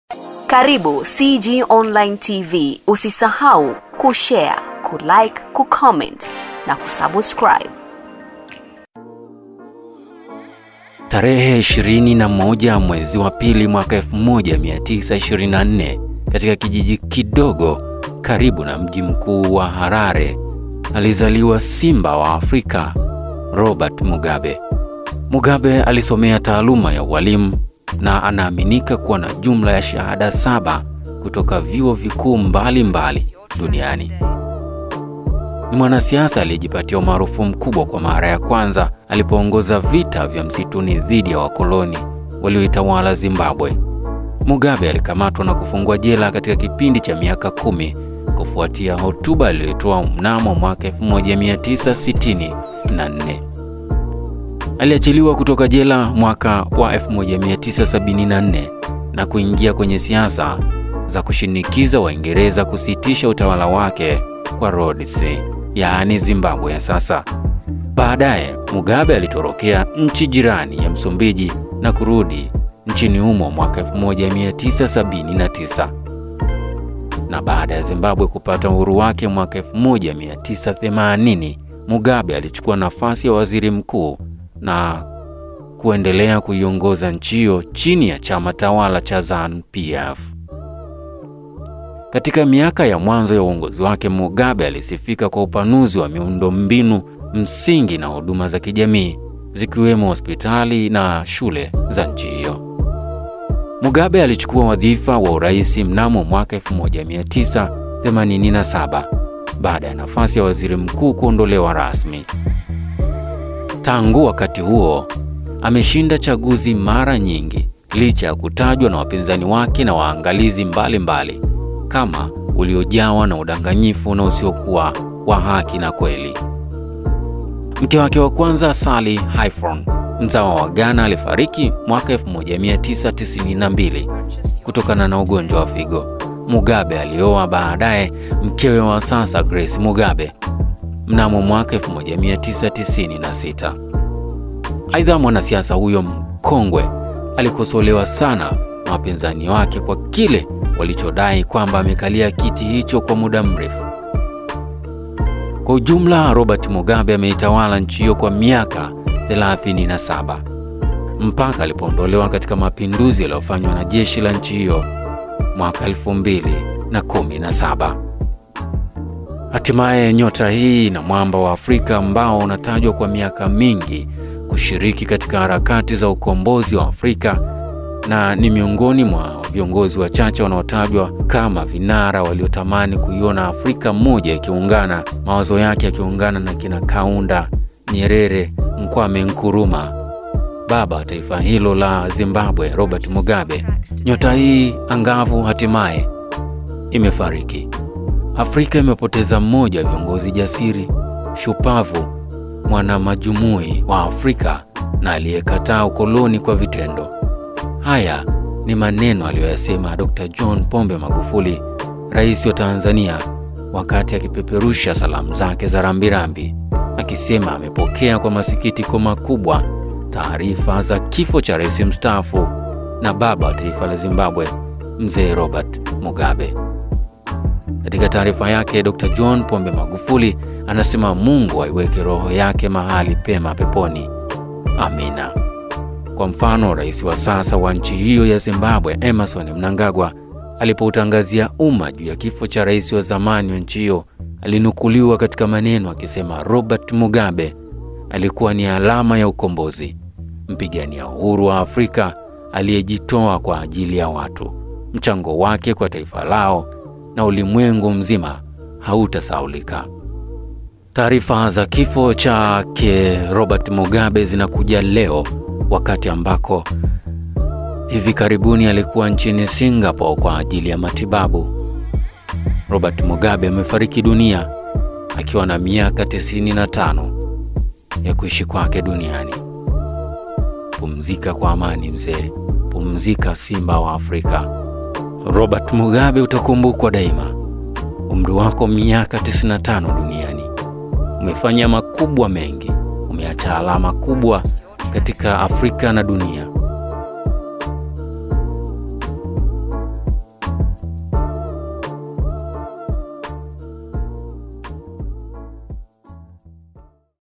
HUYU NDIO ROBERT MUGABE SIMBA WA AFRIKA,AMEFARIKI AKIWA NA UMRI WA MIAKA 95
Rais wa zamani wa Zimbabwe Robert Mugabe amefariki wakati akipatiwa matibabu Singapore. Mugabe aliyekuwa na umri wa miaka 95,aliiongoza Zimbabwe kwa takribani miongo minne.
Kifo chake kinatokea ikiwa ni karibu miaka miwili tangu aondolewe madarakani kwa kile kinachoelezwa kuwa ni mapinduzi ya kijeshi. Rais huyo wa zamani alikuwa akipatiwa matibabu kutokana na ugonjwa ambao haukuwekwa wazi, huko Singapore tangu Aprili mwaka huu.